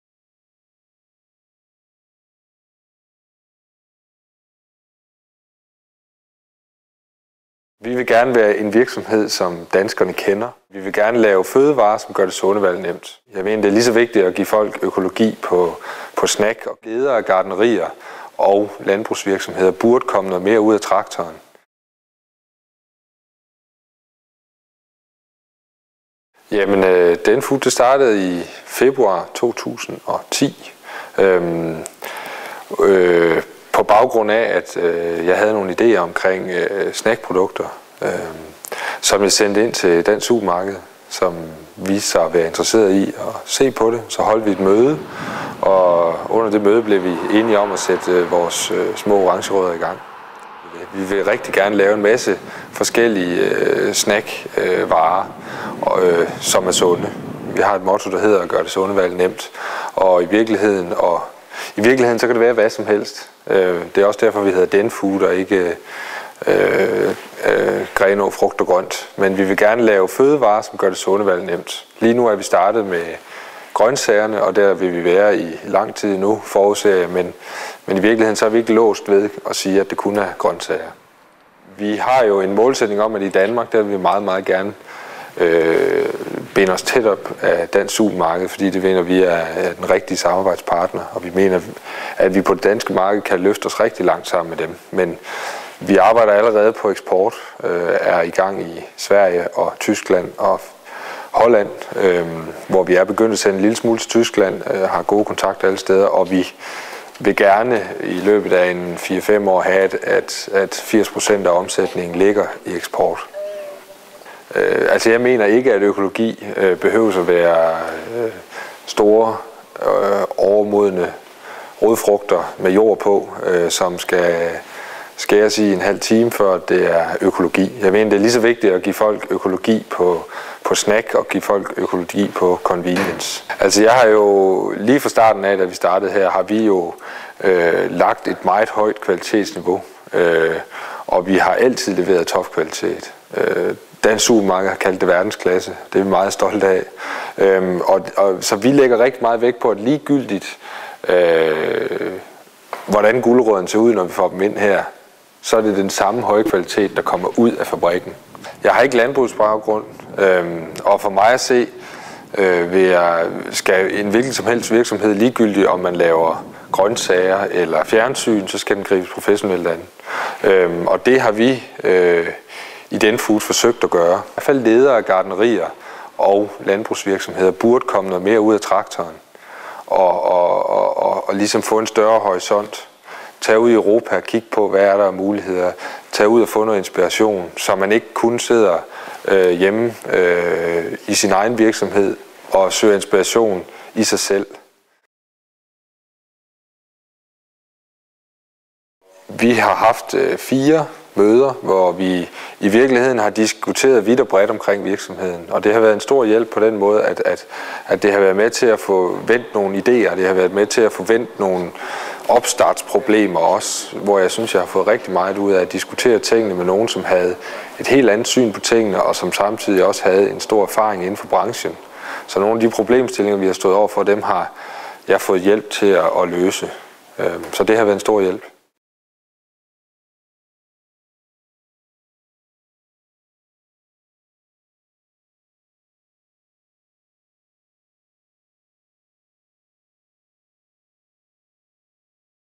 Lasse Iver Sindal om Denfood og ØkoLab
Videoklip med direktør Lasse Iver Sindal, Denfood i projektet ØkoLab. Produceret for Producentforeningen SSØRF, video af Tomas Fibiger Nørfelt, Videncentret for Landbrug, Økologi, april 2011. Finansieret med støtte fra FødevareErhverv og EU